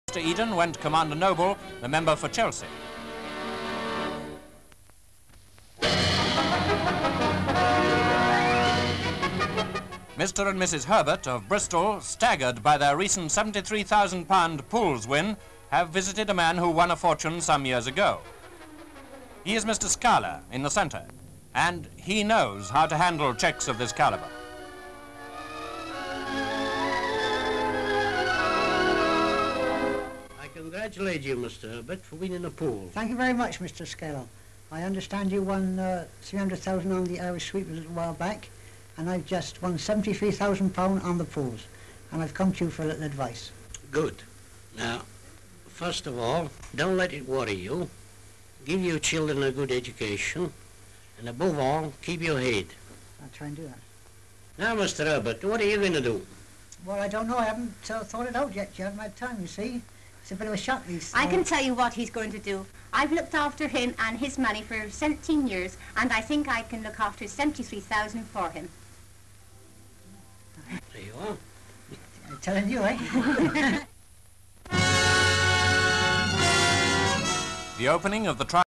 POOLS WINNER GETS ADVICE
(24 Jan 1949) Mr Herbert, with his wife, visits Emilio Scala, for advice on how to spend his £73,000 Pools win. Incidentally, Mr Scala many years ago won £300,000 on the Irish sweepstakes.